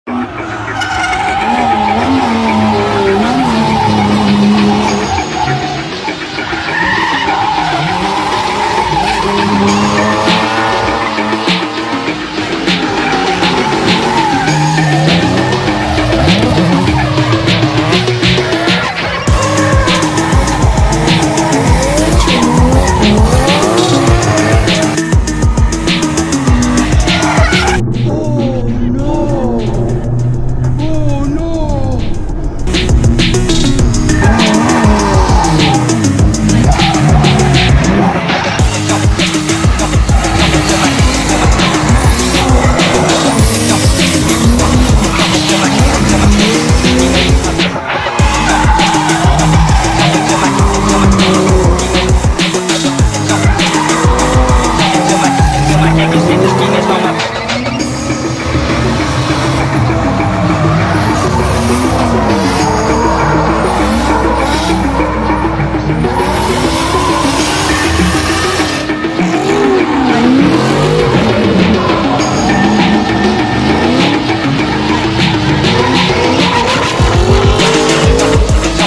Keep It Reet - Drift Championships Round 3
Few clips from Keep it Reet Drifts round 3 at Calder Park Raceway